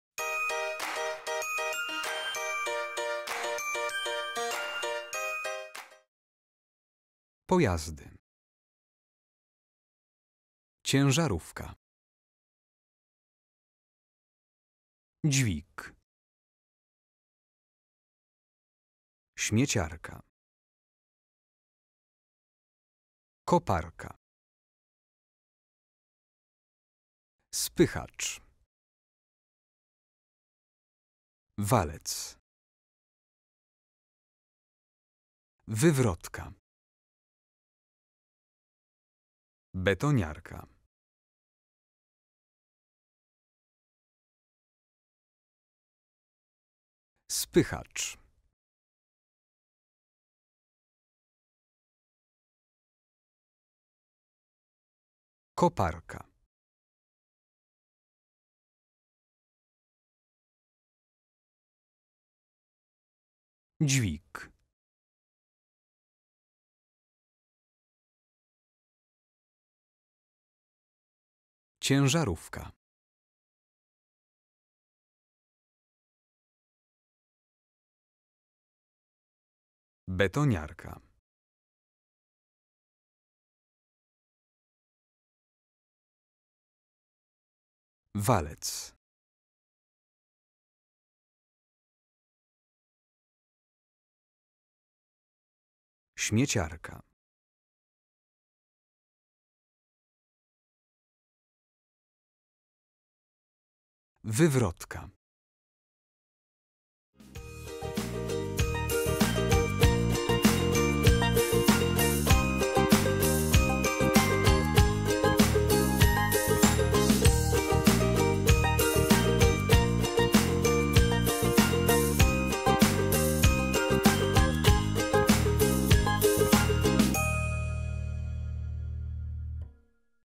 Polnisch für Anfänger - Fahrzeuge 1 (Pojazdy)
Polnisch lernen mit easy-polish.com.
Polnisch vokabeln.

► Lista słówek: ciężarówka, dźwig, śmieciarka, koparka, spychacz, walec, wywrotka, betoniarka